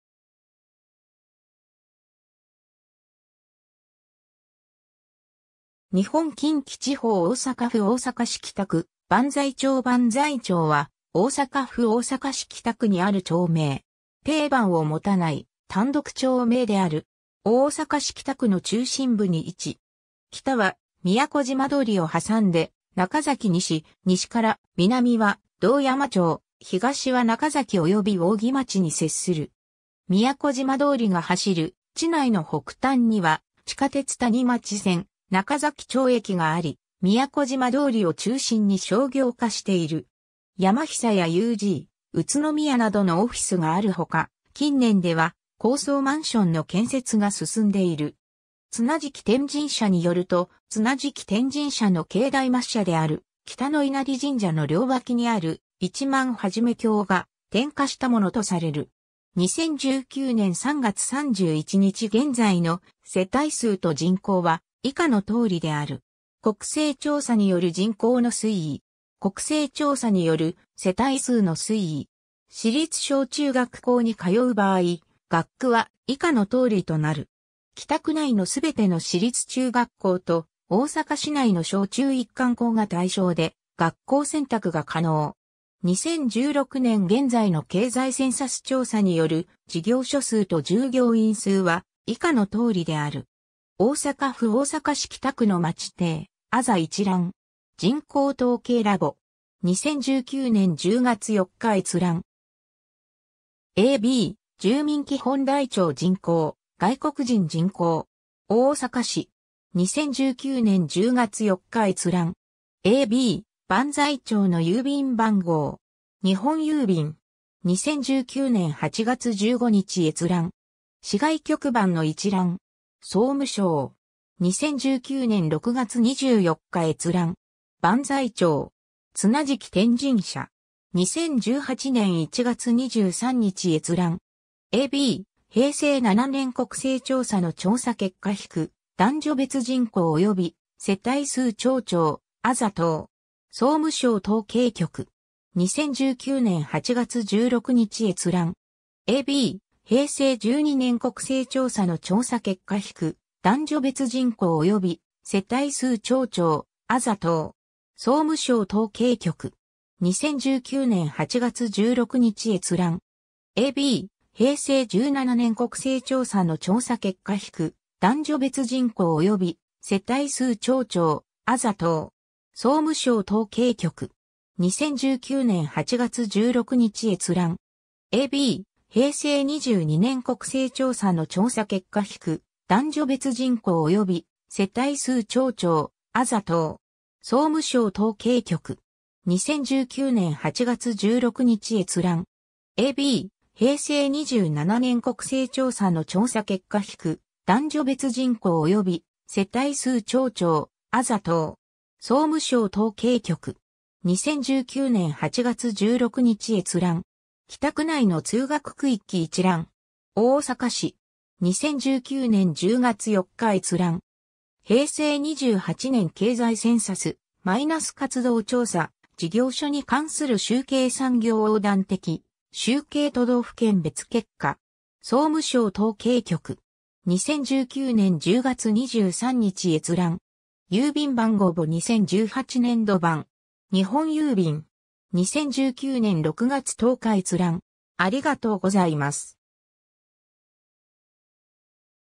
万歳町
大阪市北区の町・字
日本 」 近畿地方 」 大阪府 」 大阪市 」 北区 」 万歳町 万歳町（ばんざいちょう）は、大阪府大阪市北区にある町名。
 大阪市北区の中心部に位置。
北は都島通（大阪市道大阪環状線）を挟んで中崎西、西から南は堂山町、東は中崎及び扇町に接する。
 都島通が走る地内の北端には地下鉄谷町線中崎町駅があり、都島通を中心に商業化している。
ヤマヒサやUG・宇都宮などのオフィスがある他、近年では高層マンションの建設が進んでいる。
 綱敷天神社によると、綱敷天神社の境内末社である喜多埜稲荷神社の両脇にある「萬載橋」が転訛したものとされる。
 2019年（平成31年）3月31日現在の世帯数と人口は以下の通りである。
北区内の全ての市立中学校と、大阪市内の小中一貫校が対象で学校選択が可能（抽選を実施）。
 2016年（平成28年）現在の経済センサス調査による事業所数と従業員数は以下の通りである。
 ^ “大阪府大阪市北区の町丁・字一覧” (日本語). 人口統計ラボ. 2019年10月4日閲覧。
 ^ a b “住民基本台帳人口・外国人人口” (日本語). 大阪市 (2019年7月26日). 2019年10月4日閲覧。
 ^ a b “万歳町の郵便番号”. 日本郵便. 2019年8月15日閲覧。
 ^ “万歳町”. 綱敷天神社. 2018年1月23日閲覧。
 ^ “北区内の通学区域一覧”. 大阪市 (2018年12月6日). 2019年10月4日閲覧。
 “（ファイル元ページ）” ^ “平成28年経済センサス－活動調査 / 事業所に関する集計 産業横断的集計 都道府県別結果” (日本語). 総務省統計局 (2018年6月28日). 2019年10月23日閲覧。